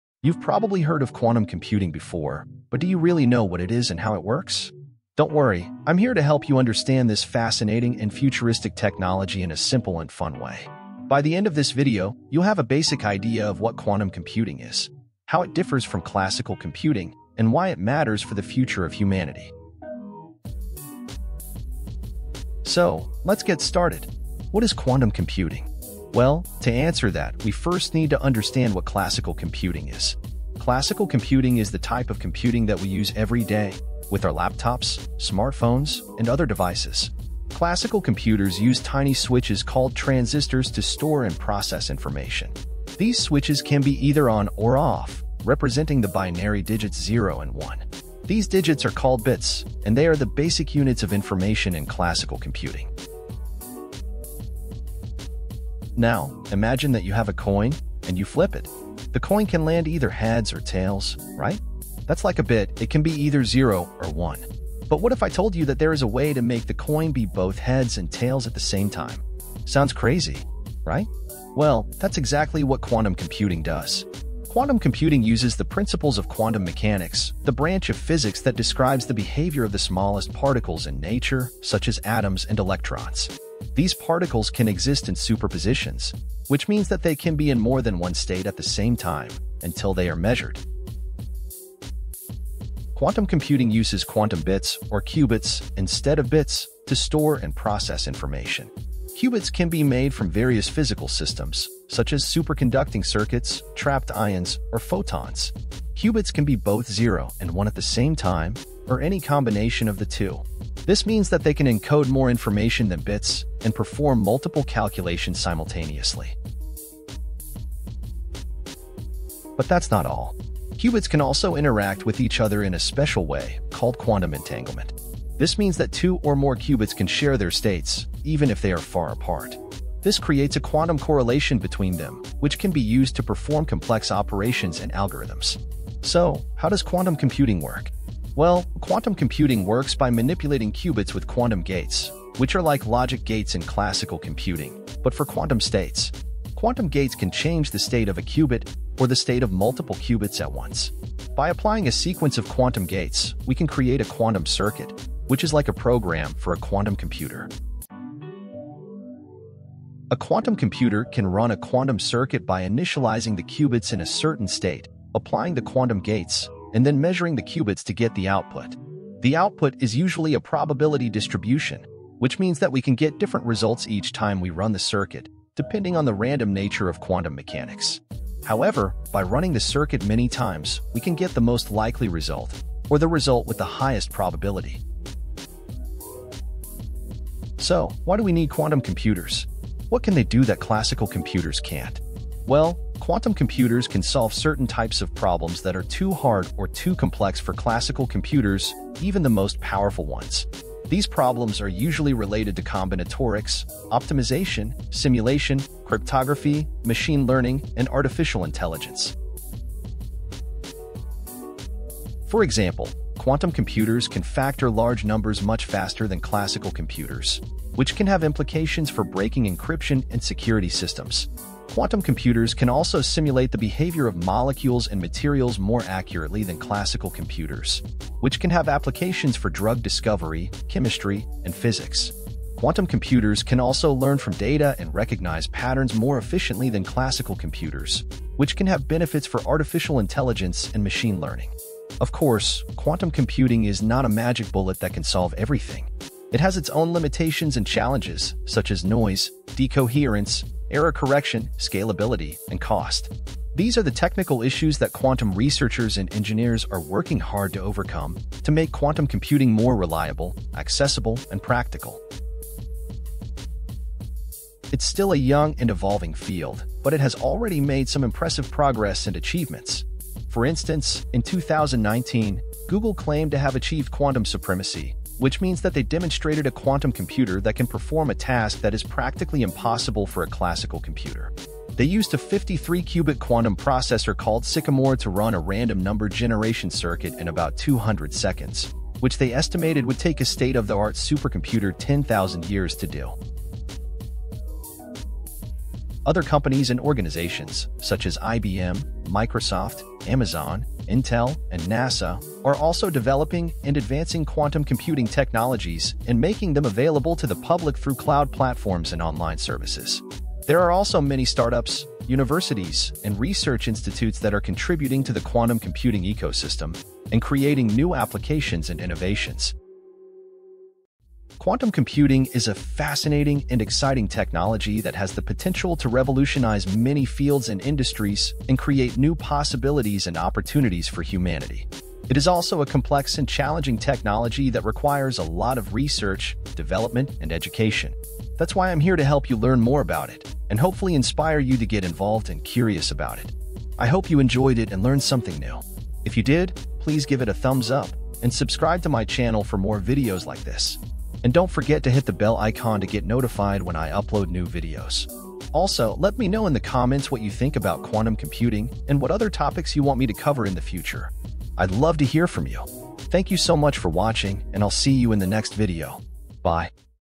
Quantum Computing Explained in 6 Minutes - The Future of Computing
Have you ever wondered what quantum computing is and how it works?

Quantum computing is a revolutionary technology that uses the principles of quantum physics to perform calculations that are impossible for classical computers. It has the potential to transform fields like cryptography, drug discovery, and artificial intelligence.
In this video, I will explain quantum computing in a simple and fun way, using examples and analogies that anyone can understand. You will learn:

What is quantum computing and how it differs from classical computing

What are qubits, superposition, and entanglement, and how they enable quantum computing

How quantum computers work, and what are quantum gates and circuits

What are the applications, limitations, and challenges of quantum computing

What are the current and future developments and achievements in quantum computing

By the end of this video, you will have a basic idea of what quantum computing is, and why it matters for the future of humanity.